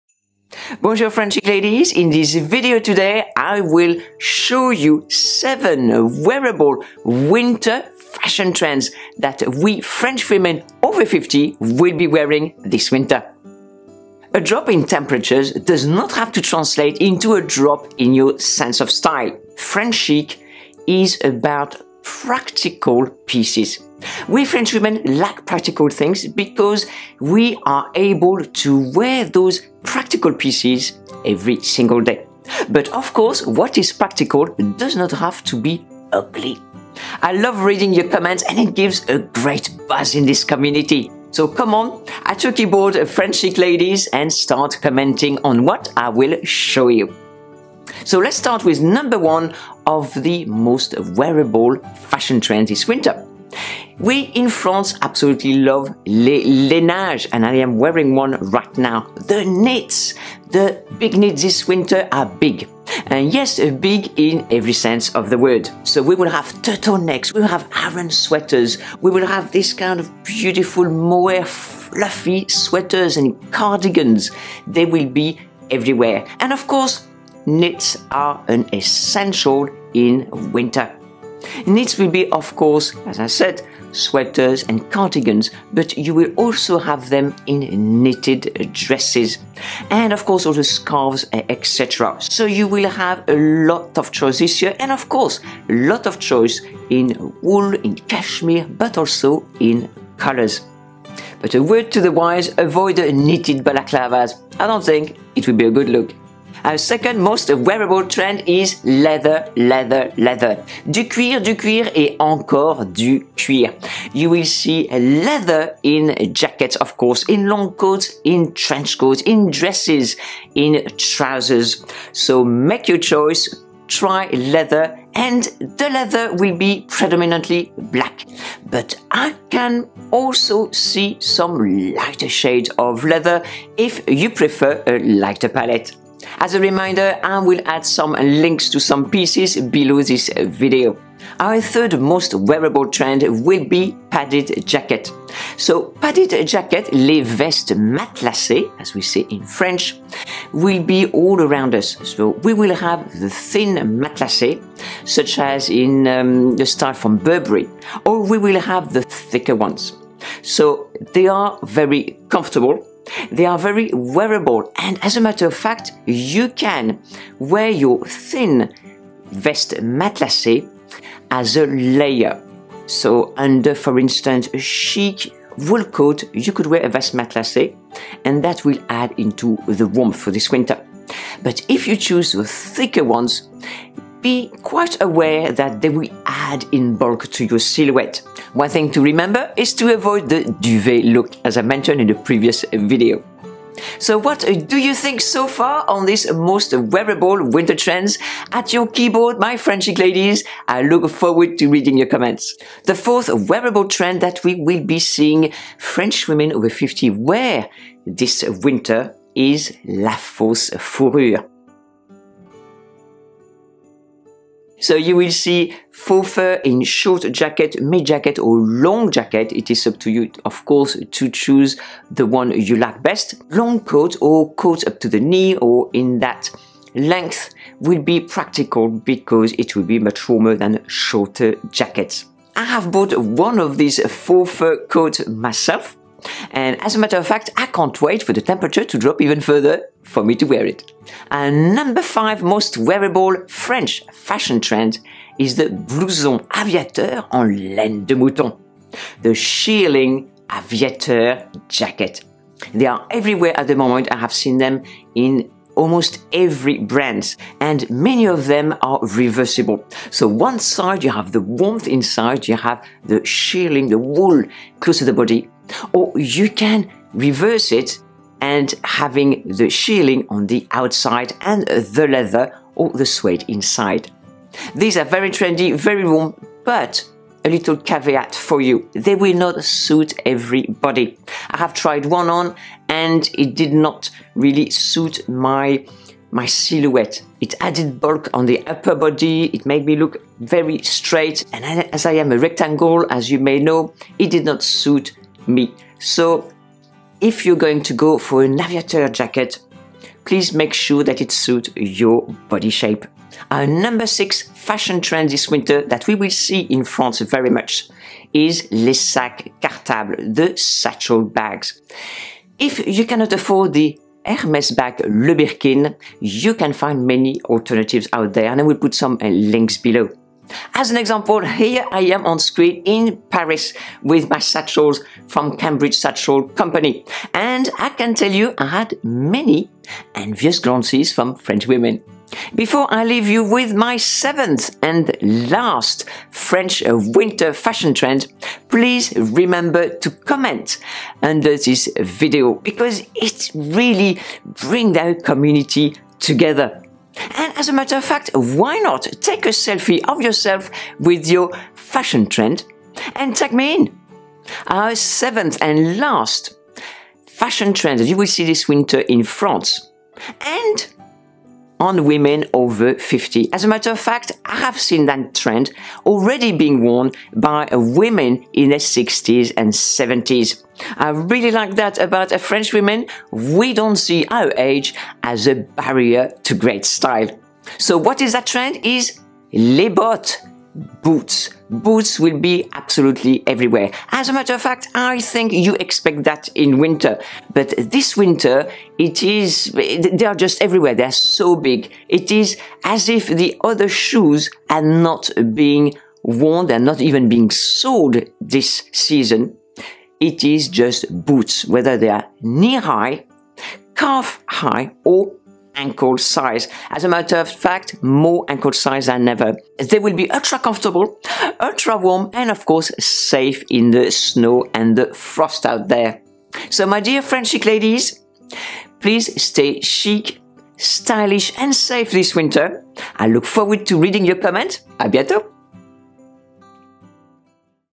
7 Wearable Winter Fashion Trends 2021 To Look Younger
In today's video I show you 7 very wearable winter fashion trends from France that are in for Winter 2021 so that you can bring some authentic French Chic Style to wherever you live this winter season.

🇫🇷 Contents of this video:
00:00 Intro
00:14 Winter Style
00:47 1 Knits
01:46 2 Leather
02:21 3 Padded Jackets
03:22 4 Faux Fur
04:00 5 Leather
05:03 6 Satchel Bags
05:54 7 Boots

🇫🇷 Read my new article -  in the latest Edition of VOUZ! Magazine - free to read online now

🇫🇷 Website and French Chic style courses by The French Chic Expert

🇫🇷 ITEMS (or similar)
KNITS
SWEATER - RALPH LAUREN
SWEATER - VERONICA BEARD
DRESS - CEFINN
CARDI - EVERLANE
SWEATER - NORDSTROM

PADDED JACKETS
JACKET - BURBERRY
JACKET - COLE HAAN
TRENCHCOAT - BARBOUR
PADDED COAT - RIVER ISLAND

FAUX FUR
SHERPA COAT - BANANA REPUBLIC
FUR COAT - GALLERY
CROP JACKET - BLANKNYC

SHEARLING BOMBER JACKET
ACNE STUDIOS
JACKET - SAM EDELMAN
ACNE STUDIOS
JACKET - BANANA REPUBLIC
JACKET - RIVER ISLAND

BOOTS
MARC FISHER
CHRISTIAN LOUBOUTIN
MICHEAL KORS
TORY BURCH

MAC Russian Red Lipstick
CHANEL Concealer
CHANEL Eyeliner
& OTHER STORIES Eye Shadow-Midnight Dahlia
BENEFIT Brow Pencil